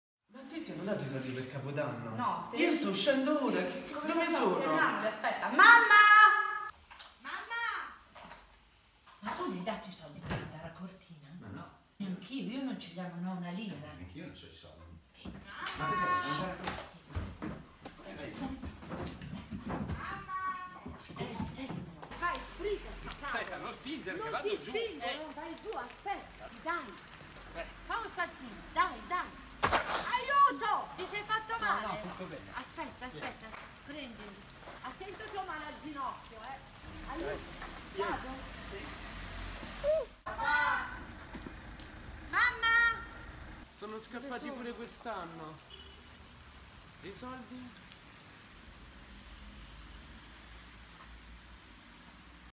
I SOLDI PER CAPODANNO sketch 009
Uno sketch ogni lunedì
fuga per non dare i soldi per capodanno

Seguite la mia linea di jeans